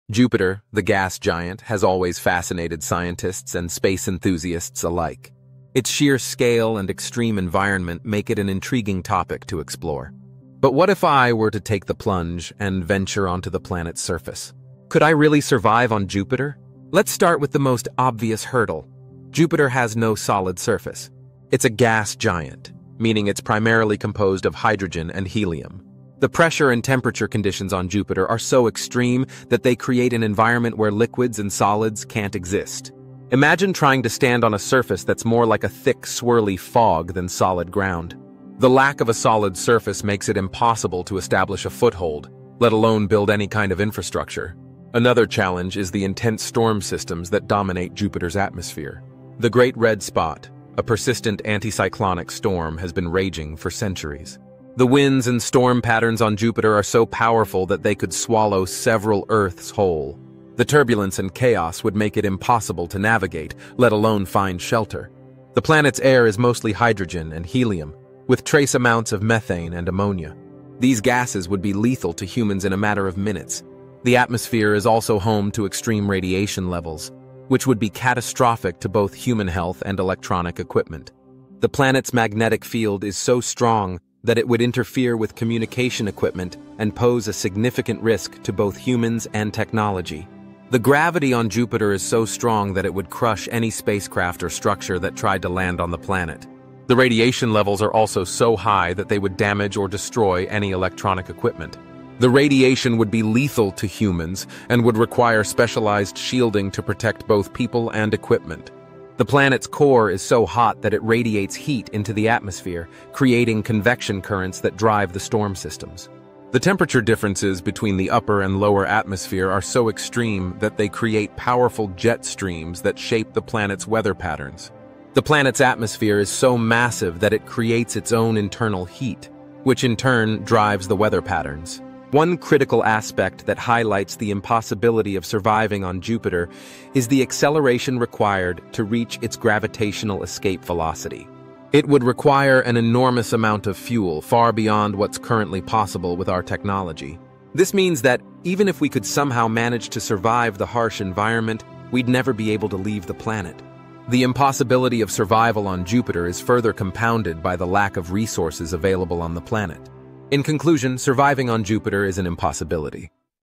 What Makes Jupiter Completely IMPOSSIBLE to Live On?
Jupiter, the largest planet in our solar system, is a gas giant known for its intense storms, extreme pressure, and scorching temperatures. But have you ever wondered, could you really survive on Jupiter? From the crushing pressure of its atmosphere to the toxic gases that make up its air, Jupiter is a hostile environment that's seemingly inhospitable to human life. In this video, we'll explore the incredible challenges of surviving on Jupiter and what it would take to make it out alive. From the extreme conditions on the planet's surface to the possibilities of finding shelter in its atmosphere, we'll delve into the fascinating science behind Jupiter's harsh environment. So, buckle up and join us on this journey to the gas giant and find out if it's really possible to survive on Jupiter.